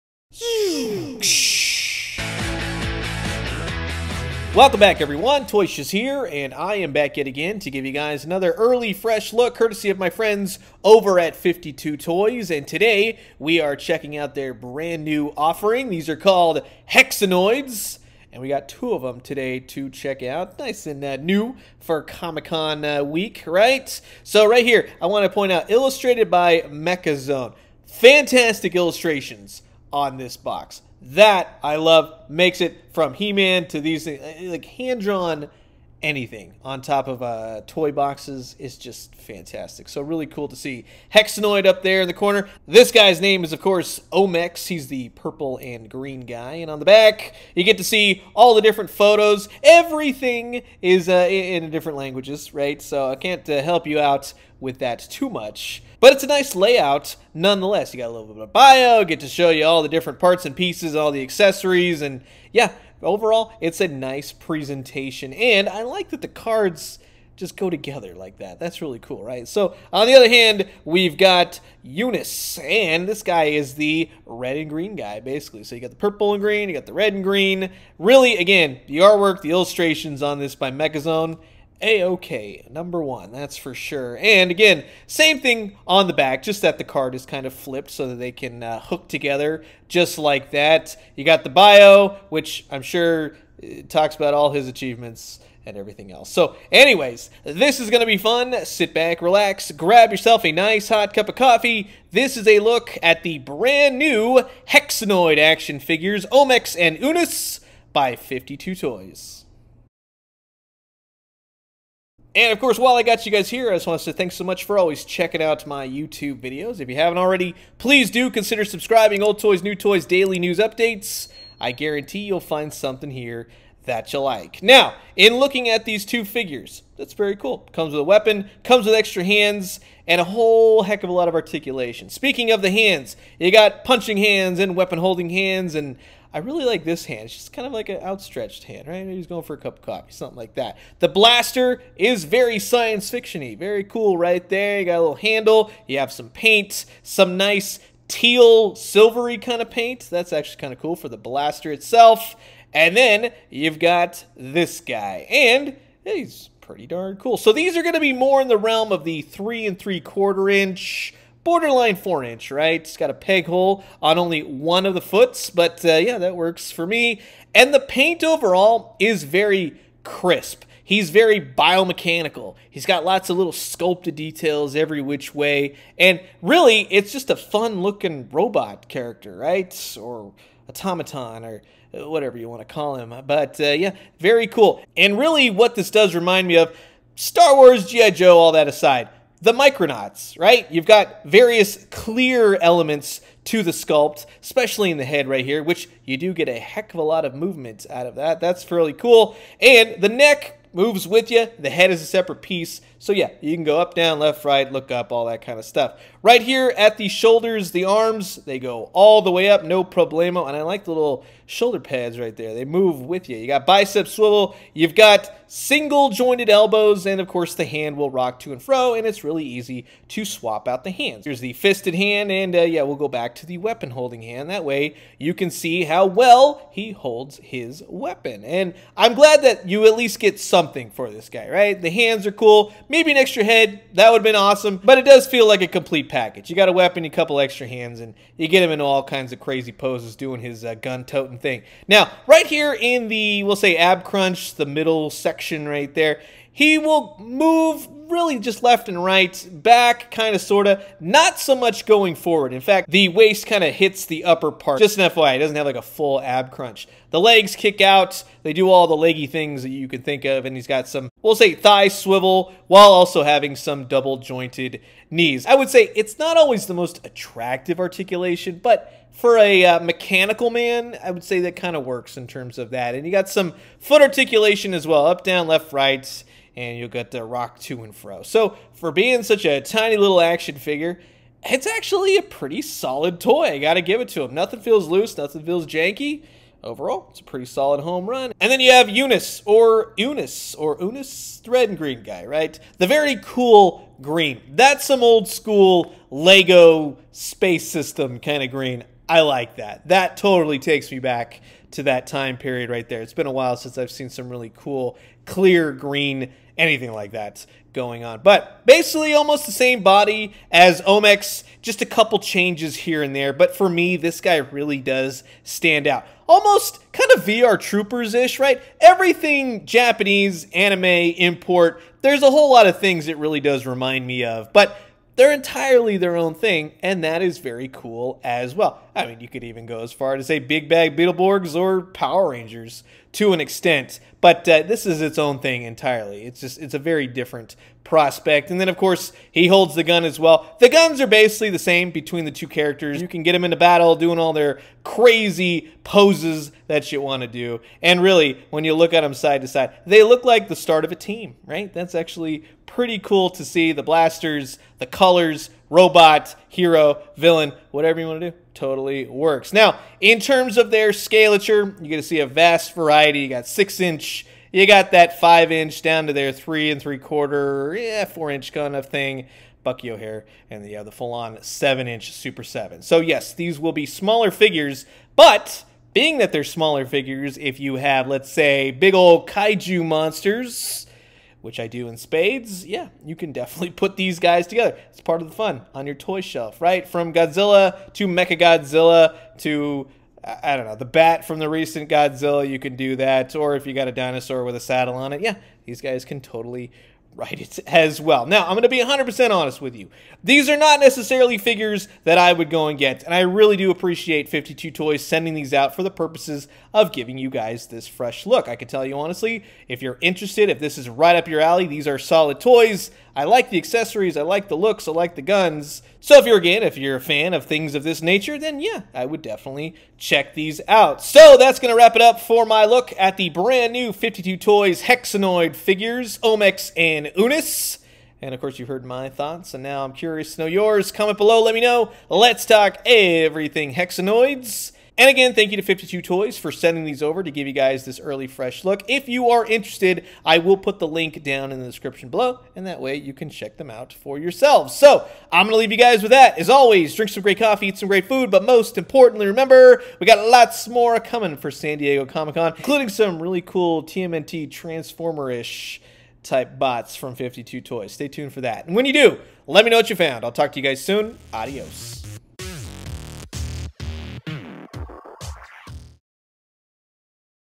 2022 HEXANOID: OMEX & UNUS | 52Toys
Everything you need to know about the brand new 52 Toys, HEXANOID figures, OMEX & UNUS!